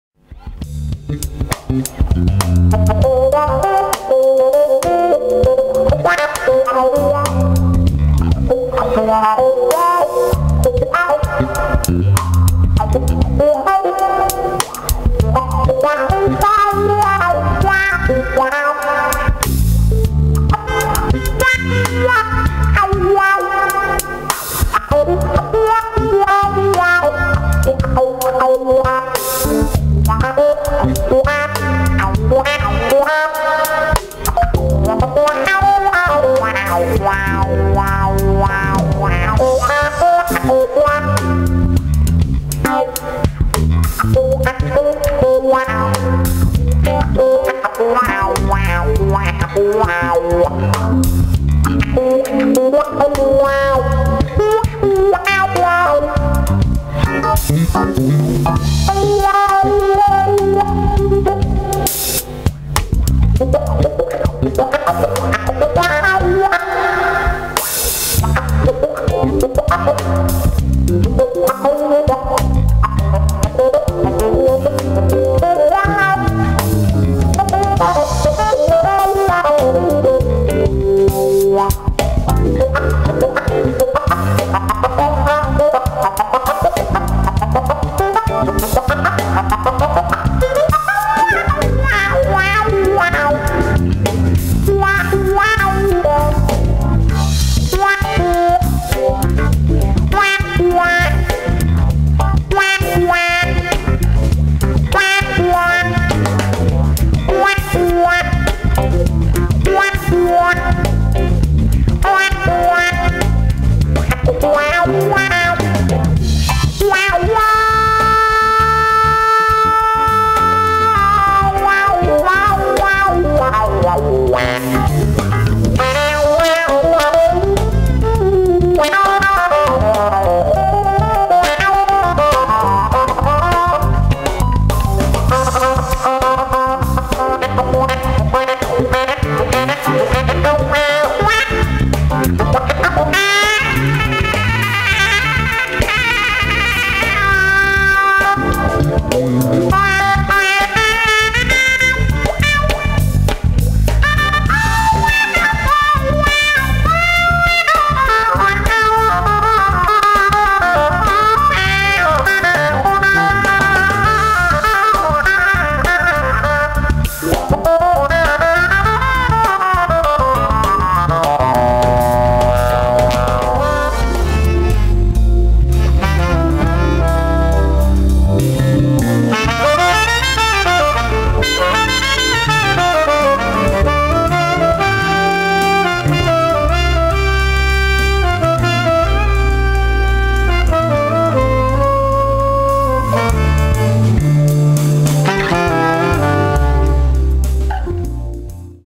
Martin Adrian mit Rainer Wind und Sebastian Motz und Micky Kamo.wmv
Durch die den Gitarrenwahah und den Oktaver kann man dem Saxophon hier selten schöne Klänge entlocken. Die hervorragenden Musiker sind: Rainer Wind am Bass, Sebastian Motz am Keyboard und Micky Kamo am Schlagzeug.